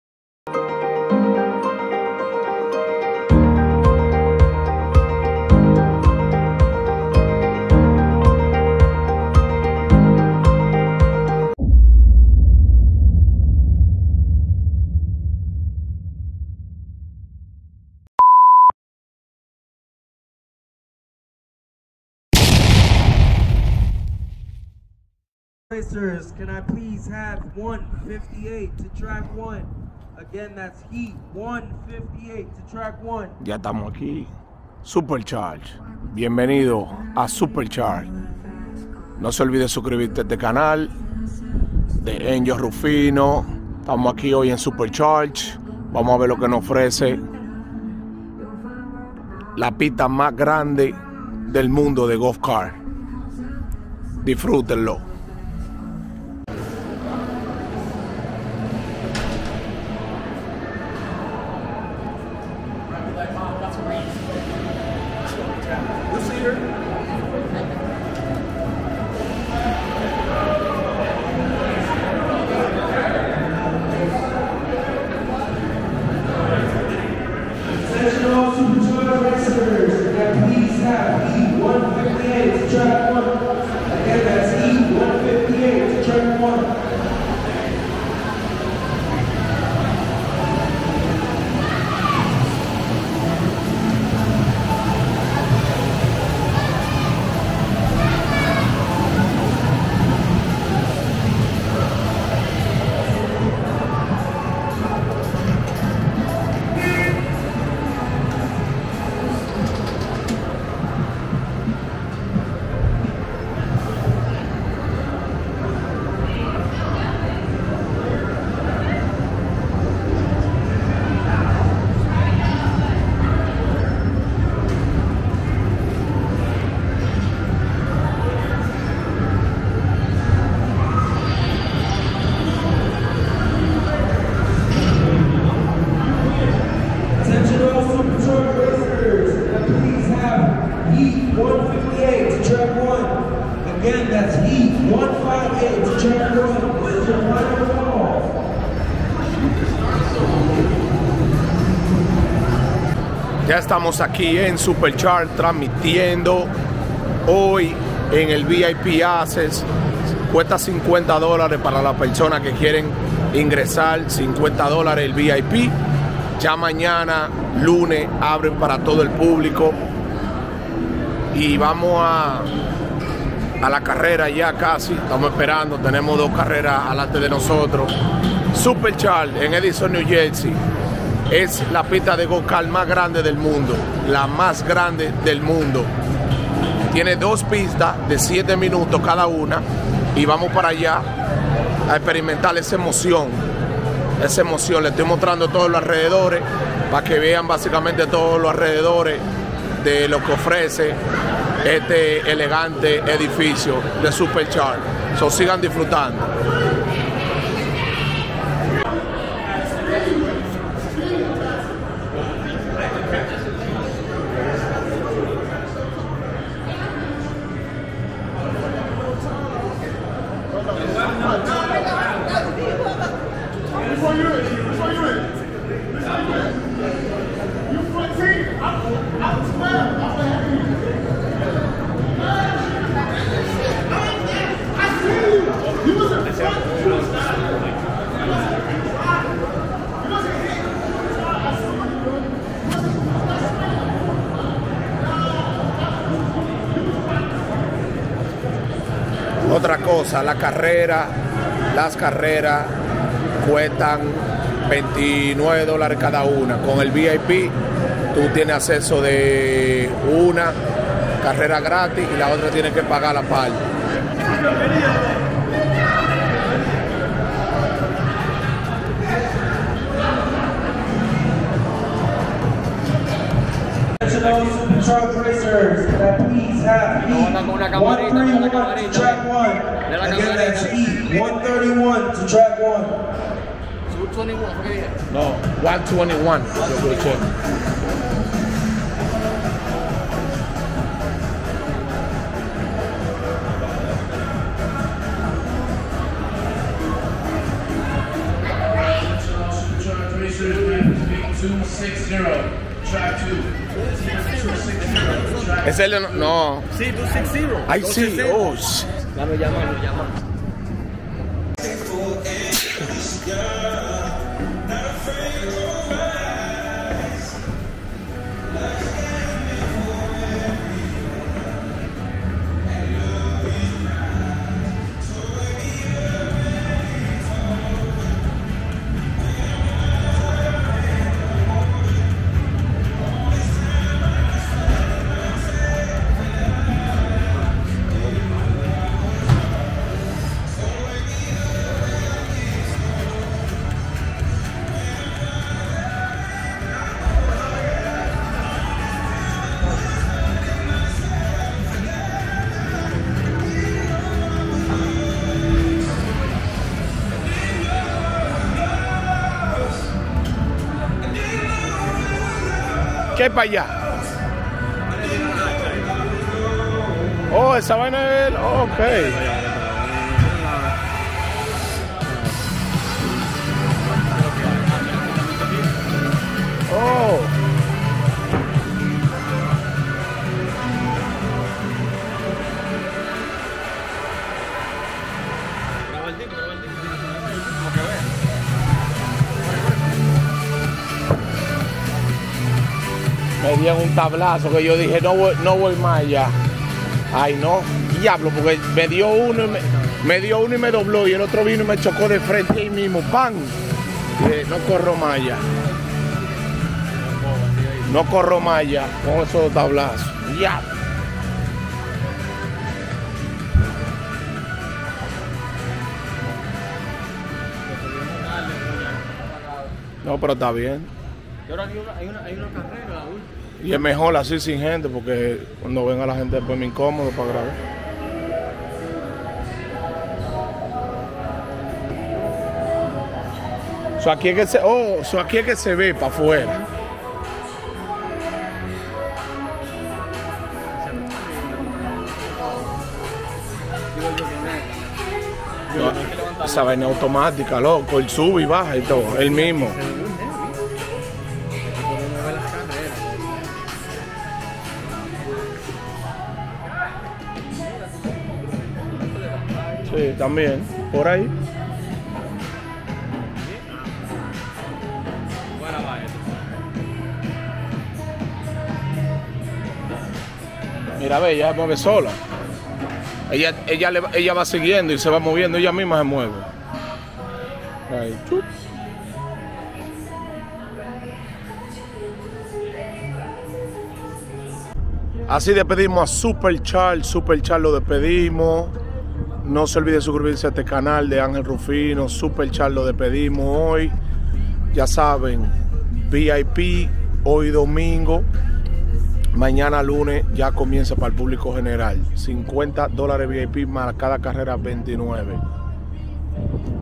LA PISTA DE GO KART MAS GRANDE DEL MUNDO EN EDISON NEW JERSEY!
en New Jersey supercharged Edison